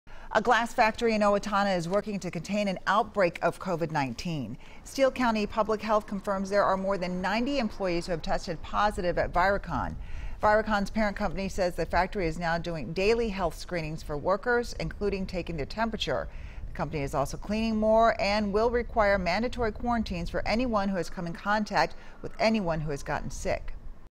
91 Workers Test Positive For COVID-19 At Viracon
A glass factory in Owatonna is working to contain an outbreak of COVID-19, Amelia Santaniello reports (0:24).